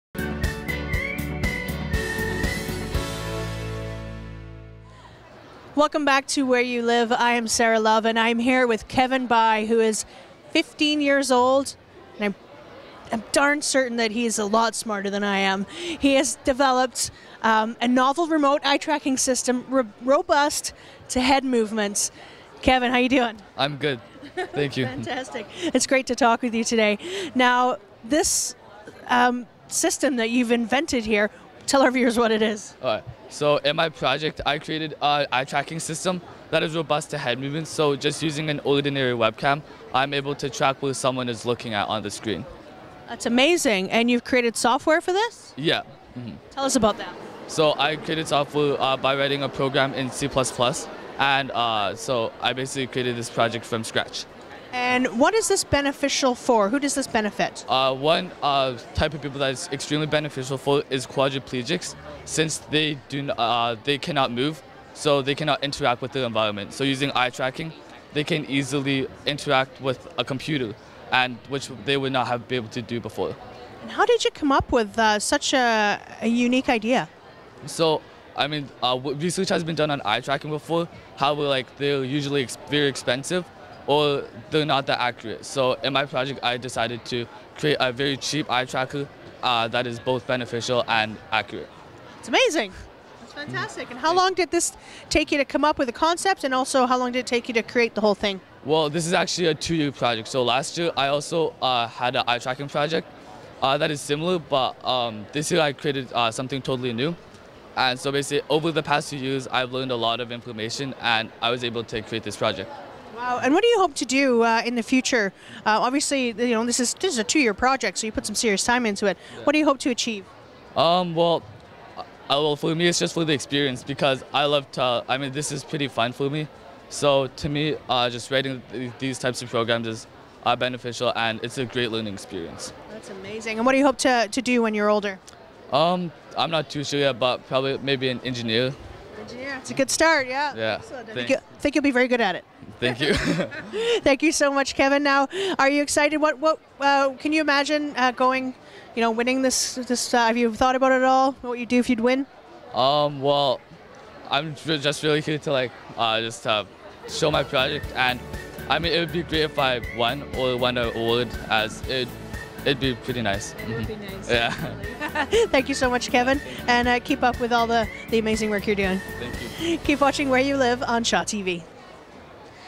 Where You Live - Robust Eye Tracking System
Kevin Bai created a novel remote eye tracking system robust using just an ordinary webcam to track what someone is looking at on a computer screen for the N.W.O. Regional Science Fair.